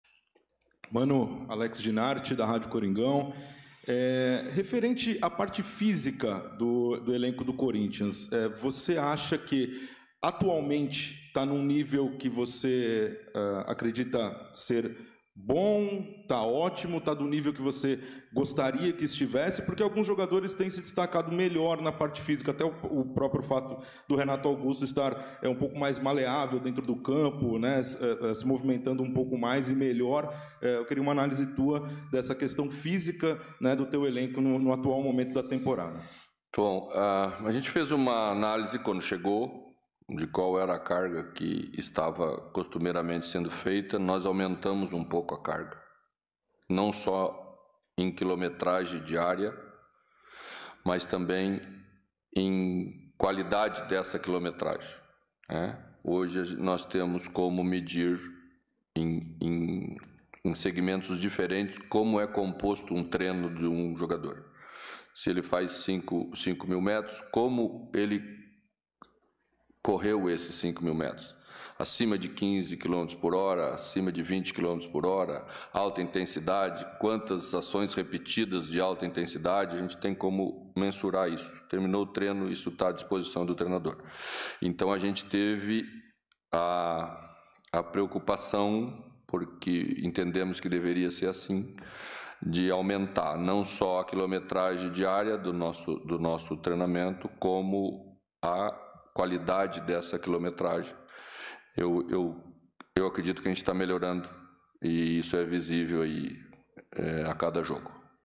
"Aumentamos a carga de treino e a melhora é notória" - Mano Menezes - Corinthians 1x1 Santos
Mano Menezes comenta sobre as mudanças físicas que teve que implementar no Corinthians.